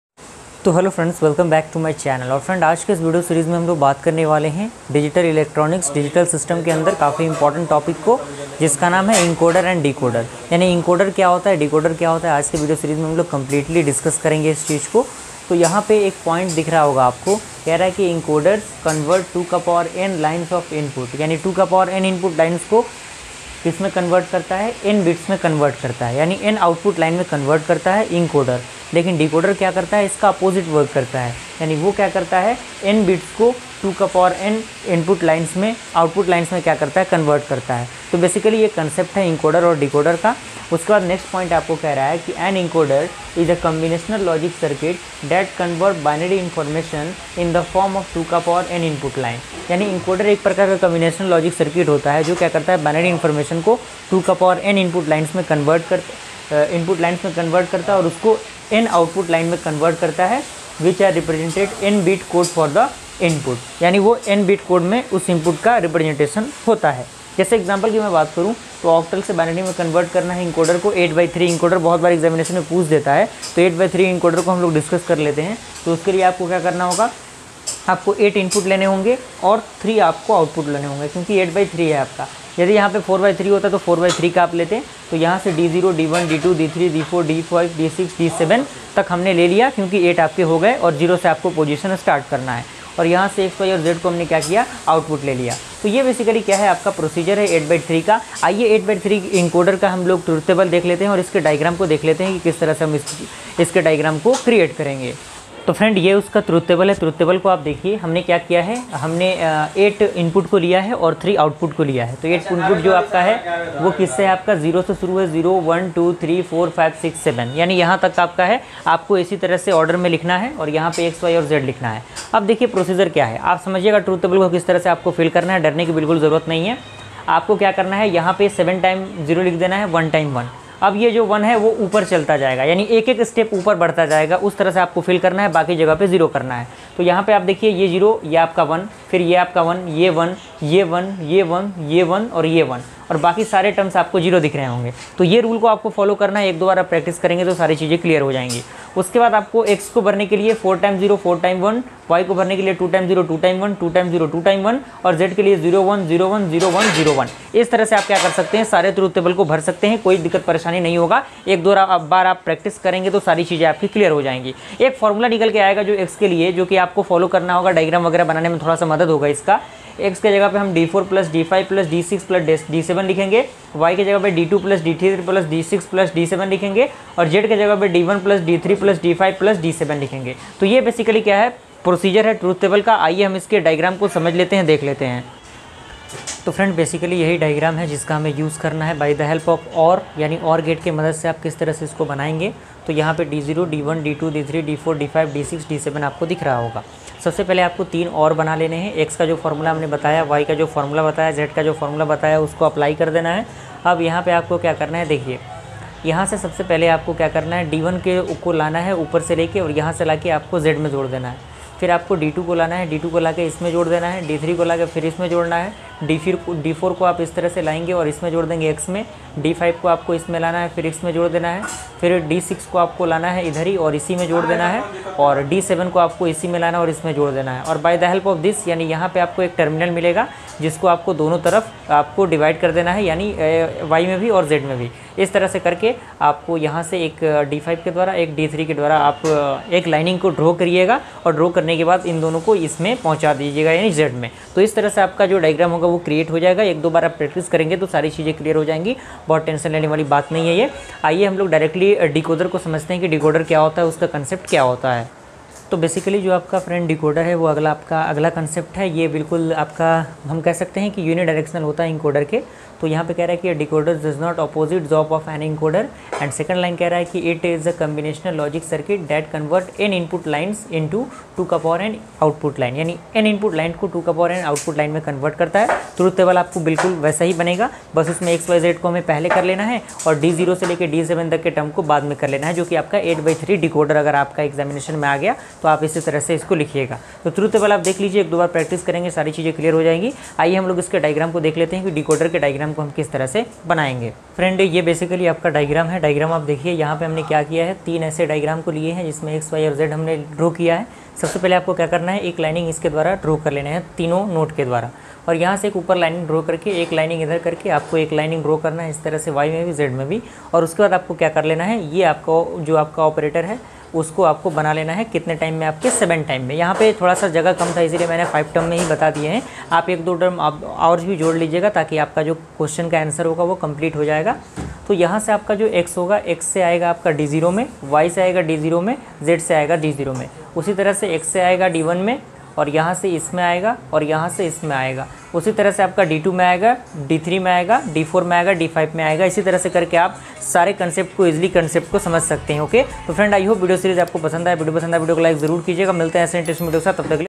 Encoder & Decoder | Decoder in Digital System | Digital Electronics
In this video We will discuss about the concept of encoder and decoder with proper explanation and examples.